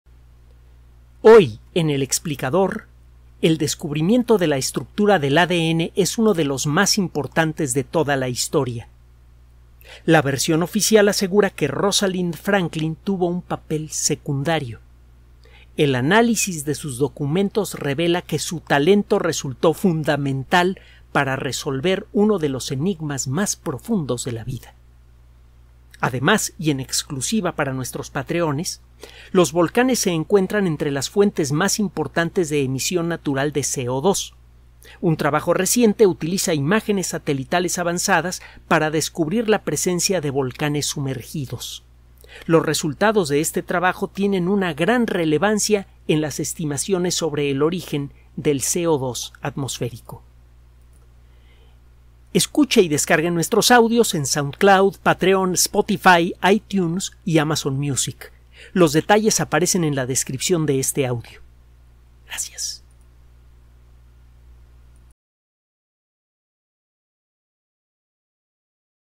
Lo de Hoy 2023/04/25. El Explicador. Cápsula.
El descubrimiento de la estructura del ADN es uno de los más importantes de toda la historia. La versión oficial asegura que Rosalind Franklin tuvo un papel secundario. El análisis de sus documentos revela que su talento resultó fundamental para resolver uno de los enigmas más profundos de la vida. Además y en exclusiva para nuestros Patreones: Los volcanes se encuentran entre las fuentes más importantes de emisión natural de CO2. Un trabajo reciente utiliza imágenes satelitales avanzadas para descubrir la presencia de volcanes sumergidos. Los resultados de este trabajo tienen una gran relevancia en las estimaciones sobre el origen del CO2 atmosférico. Los detalles aparecen en la descripción de este audio... Gracias!!